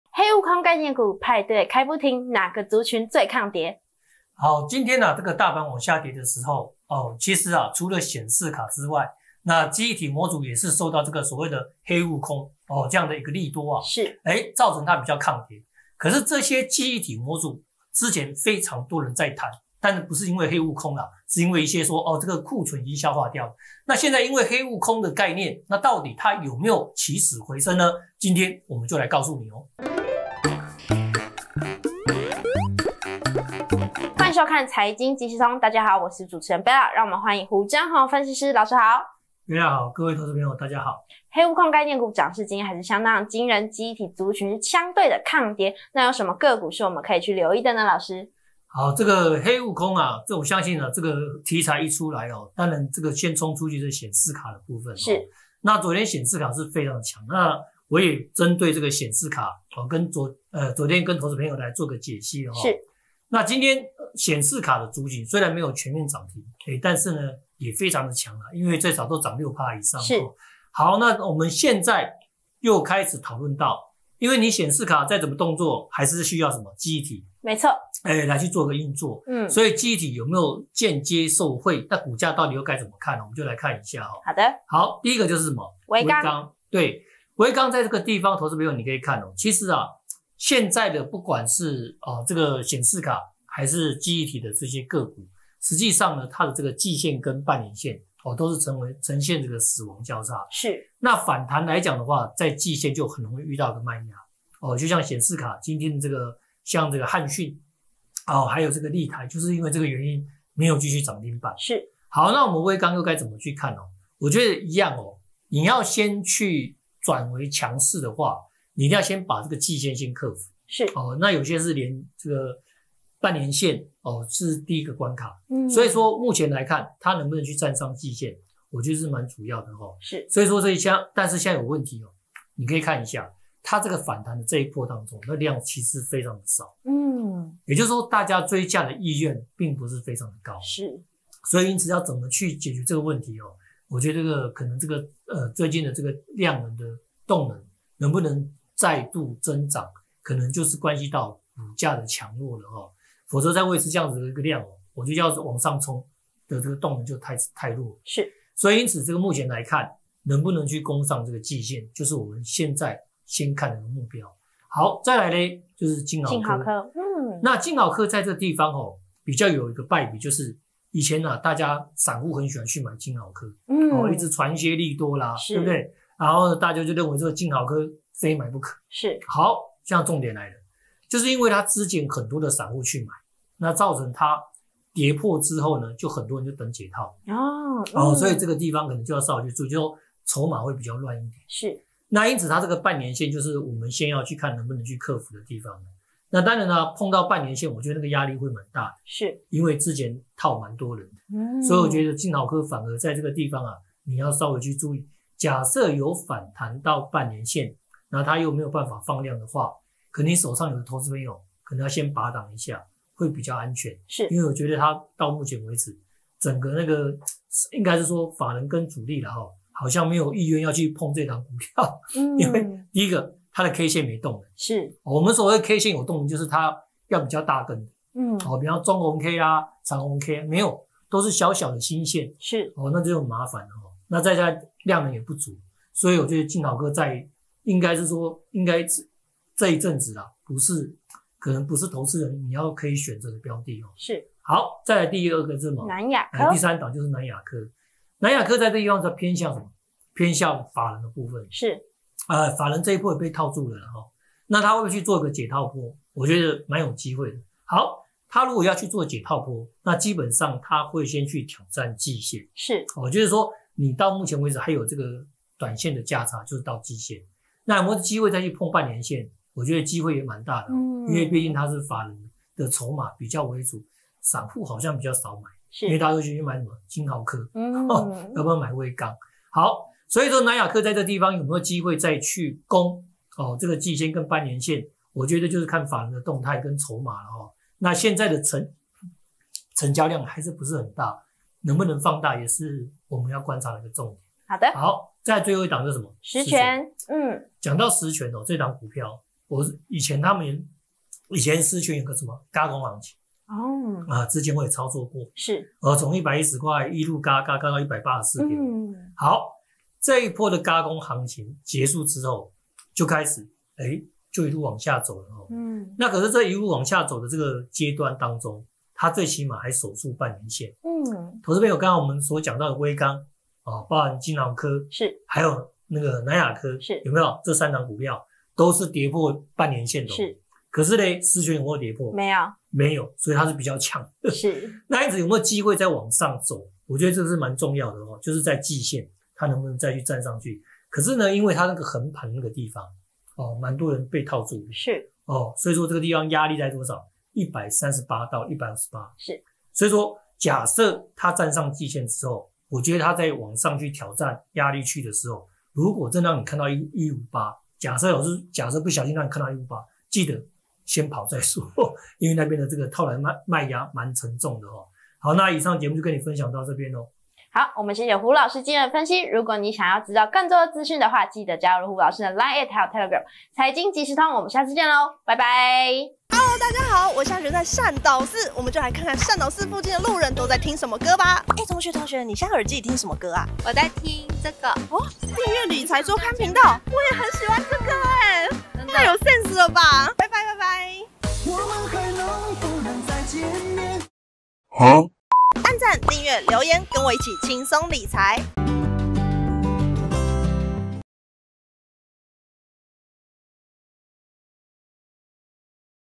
財金即時通-20240827／記憶體模組迎暖流 黑悟空概念股派對開不停？
黑悟空概念股今日漲勢續強🔥
但其中最抗跌的是記憶體族群🎮
哪些個股最值得投資朋友押寶呢？🫣
趕快看完胡老師怎麼說❗
🔥開課啦！更多財經消息都在【股市同學會】
🔥顯卡股狂飆衝漲停！一隻猴子救台股？

官方LINE@：@moneyweekly